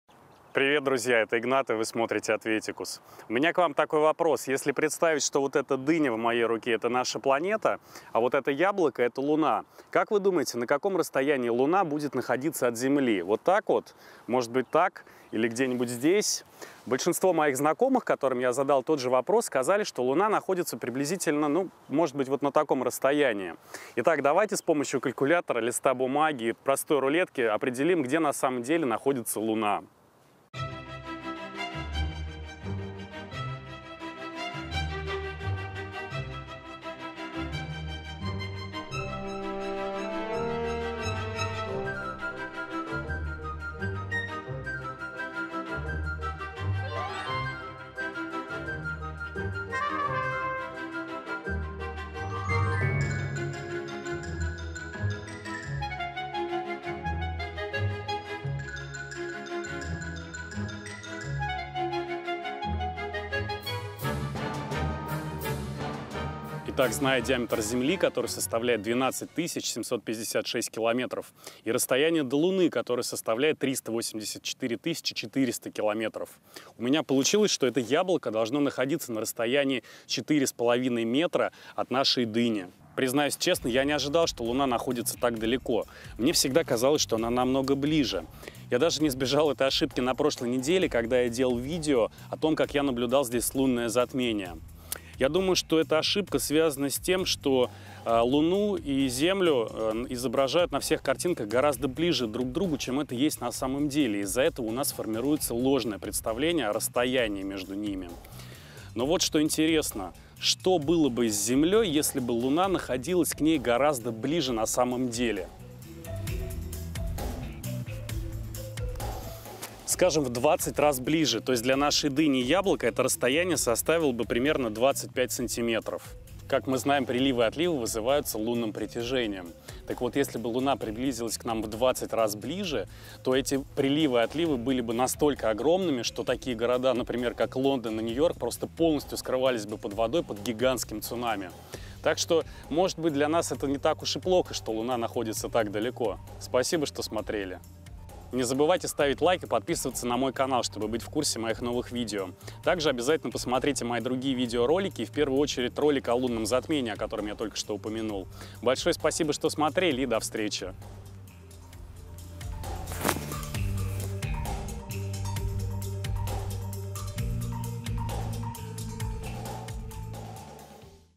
Как Далеко Луна От Земли?
Как далеко от Земли до Луны? Многие из нас не представляют, где на самом деле проходит орбита Луны вокруг нашей планеты. А что было бы если бы луна оказалась намного ближе?
Если вы хотите помочь этому каналу развиваться и делать более интересные видео, вы можете оказать финансовую поддержку

Labeled for commercial reuse with modification